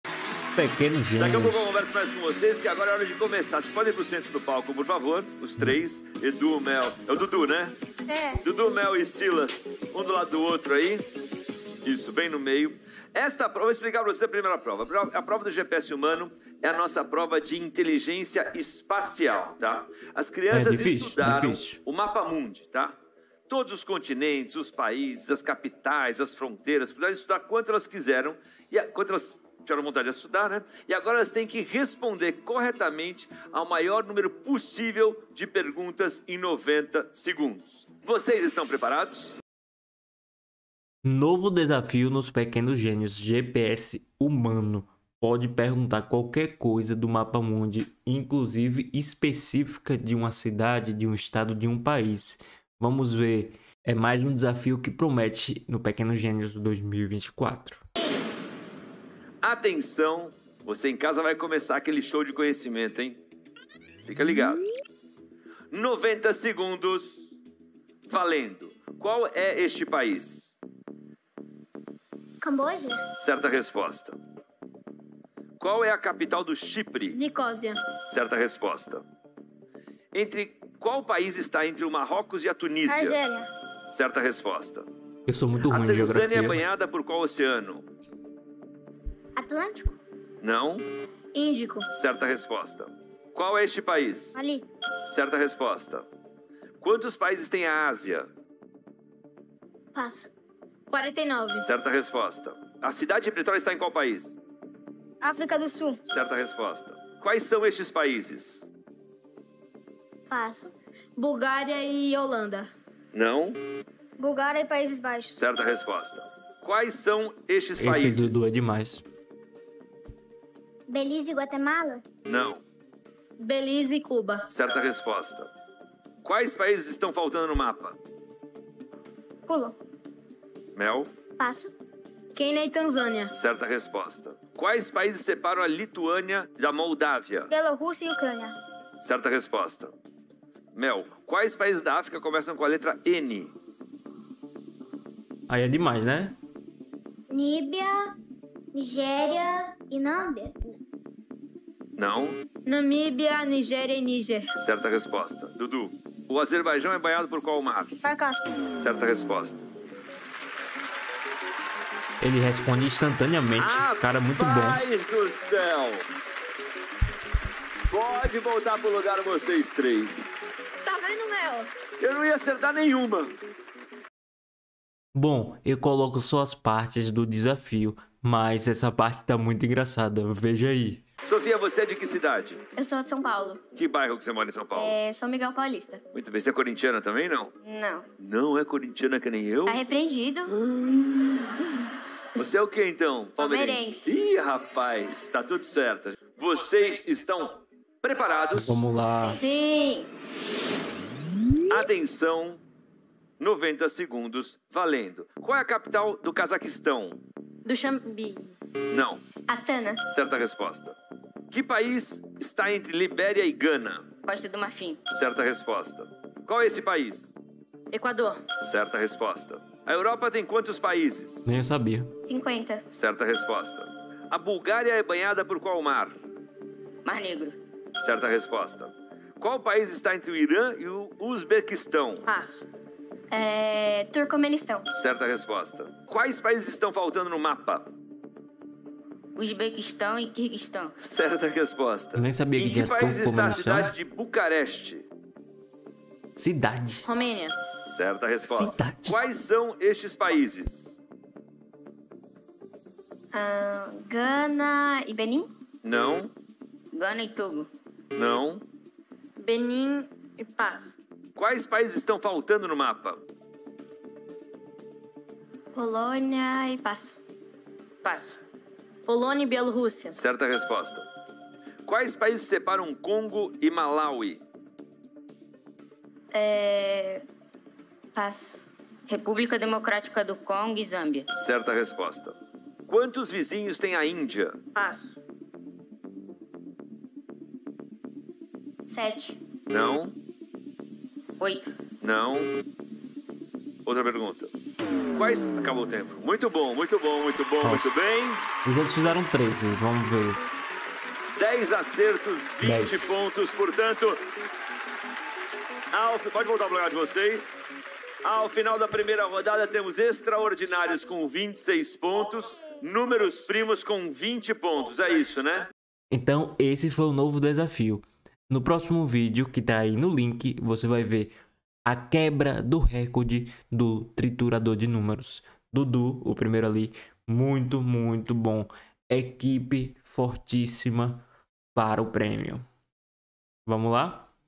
Pequenos gênios 2024 - novo desafio!
Essa é a nova fase mais desafiadora dos pequenos gênios, perguntas muito difíceis e vastas que versam sobre a geografia mundial! A cada edição vai ficando mais avançado o jogo!